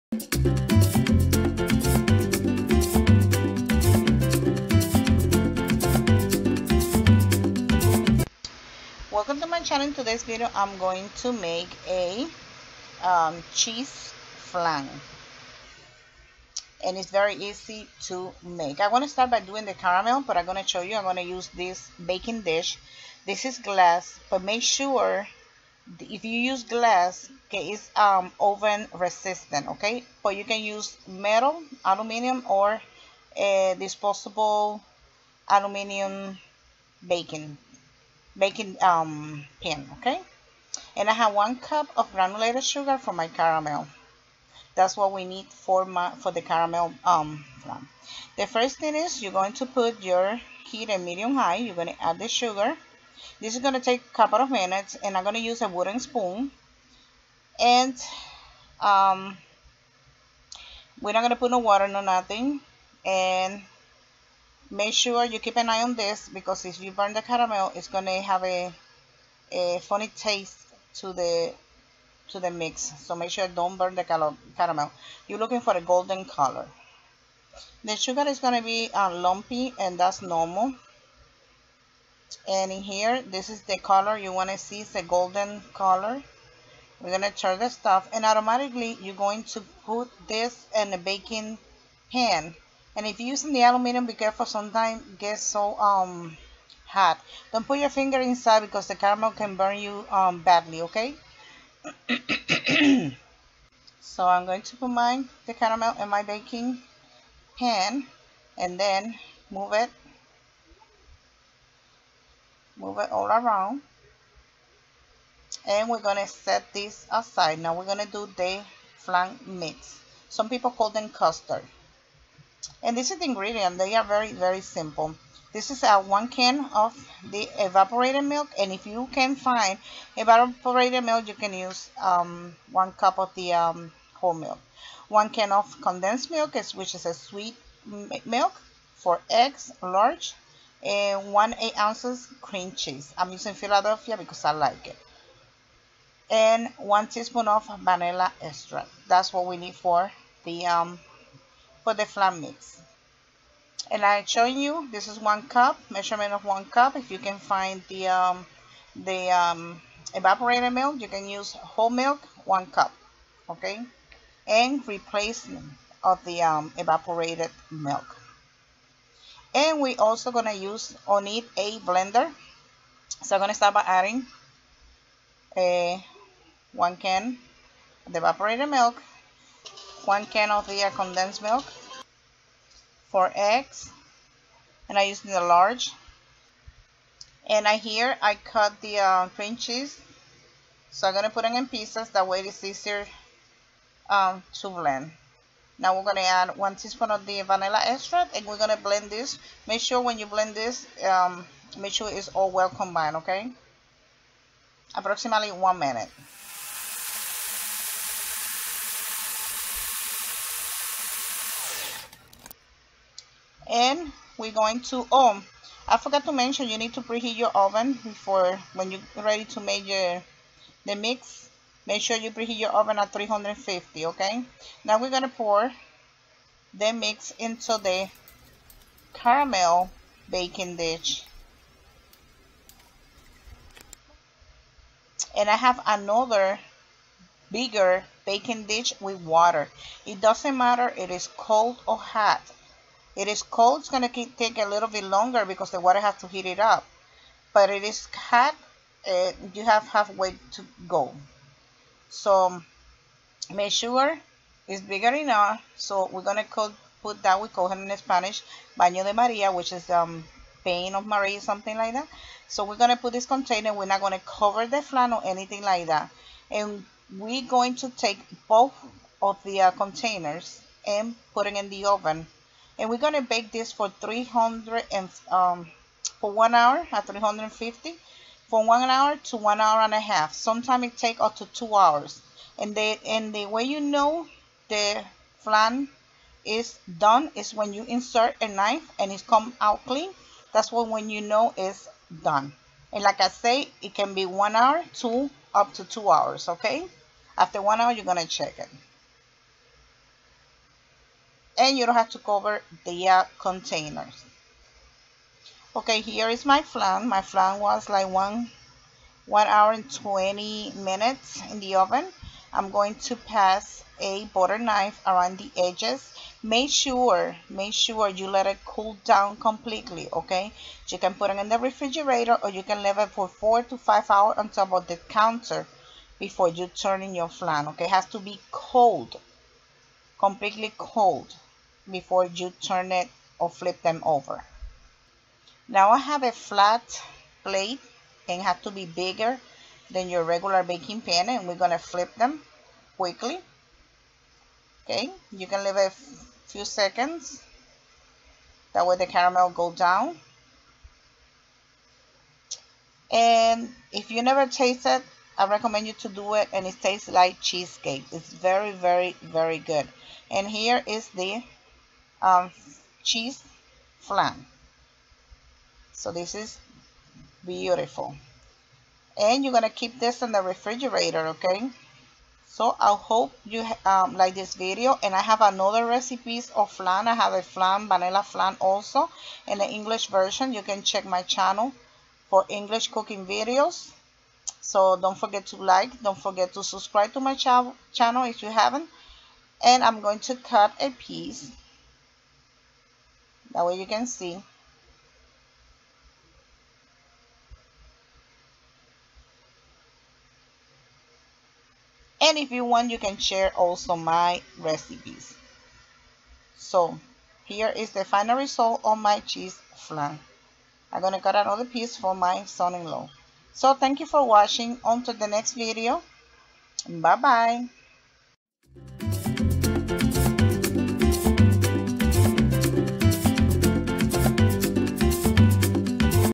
Cheese Flan Step by Step .very easy to make
Ingredients for Caramel
1 Cup granulated sugar
Ingredients
1 can Evaporated milk or 1 cup whole milk
1 can of condense milk
4 large eggs
1 8 oz  cream cheese
1 teaspoon vanilla extract
blender